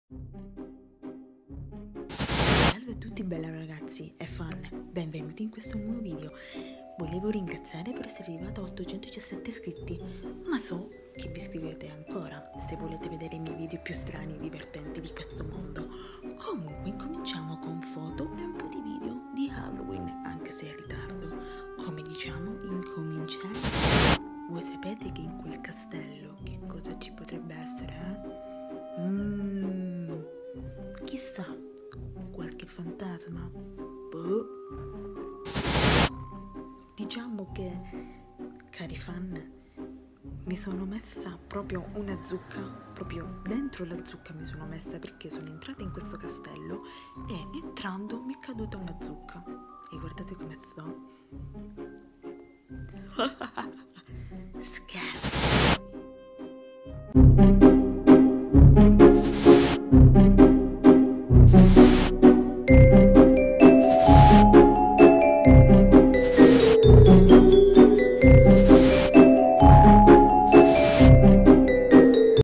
Speciale Halloween| dony sala
Scusate per il carricamento in ritardo di HALLOWEEN siccome praticamente ho dovuto fare tantissime foto stranissime per HALLOWEEN ed naturalmente sapete che dony ai vestiti ed alle smorfie che fà nn riesce a stare 1000 like per Cuesto video stranissimo di HALLOWEEN commentate ed iscrivetevi se arriverò a 100000 iscritti farò uno spettaciale se arriverò grazie al vostro aiuto           Facebook= dony sala                                                                        Instragram= dony sala                                                                   Snapchat= dony sala